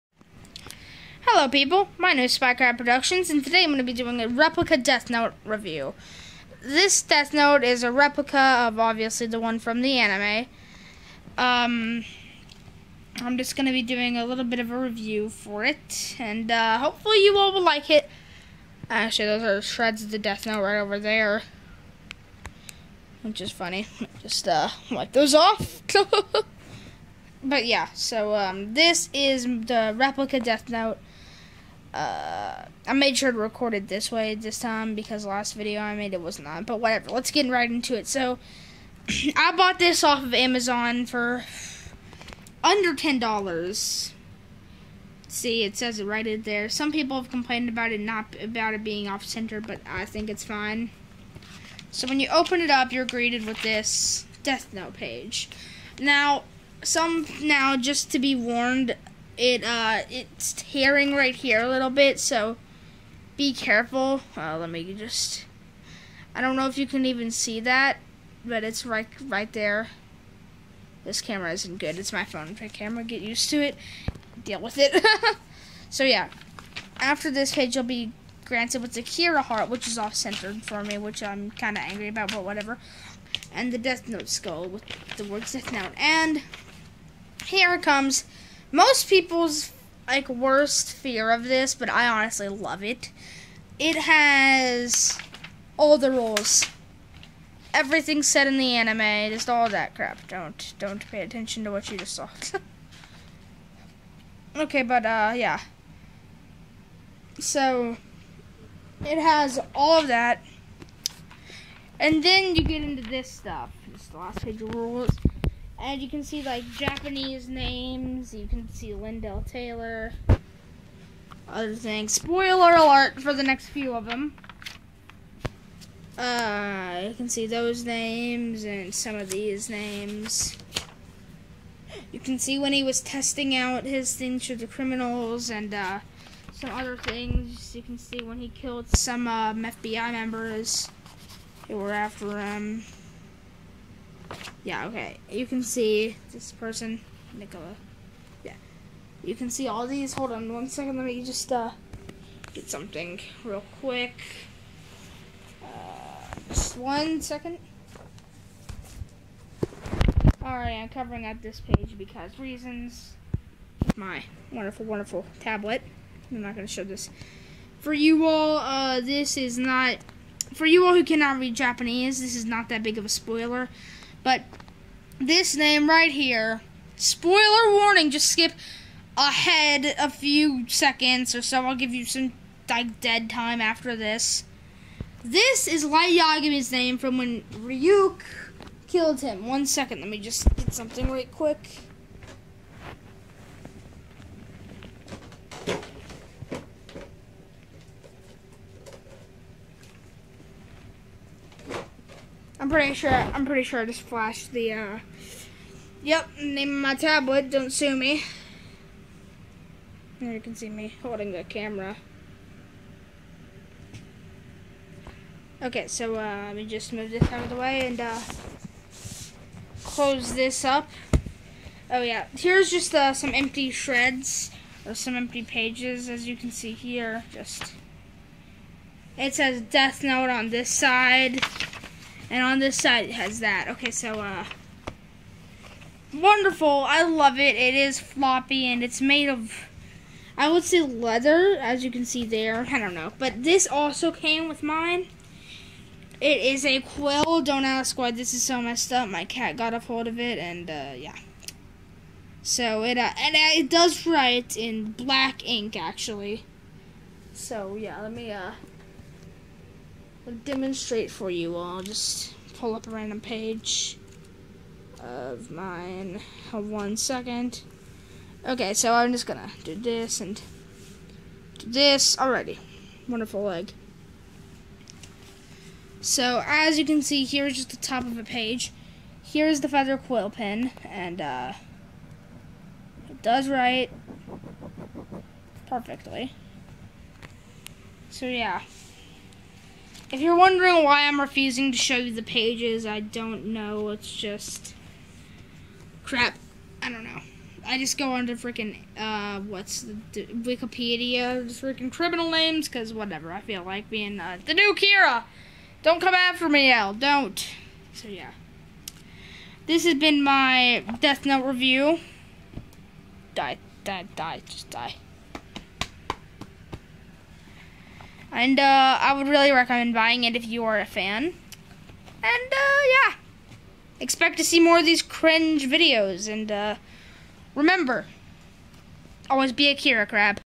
Review: Replica Death Note notebook & Quill pen
I figured well I don't have decent recording software I should do something in the meantime to post so I decided to do a replica Death Note review because I like you all and I like it and I think you should buy it buy it now NAO hahahahahaHAHAHAHAHA IM A MURDERER CANT YA TELL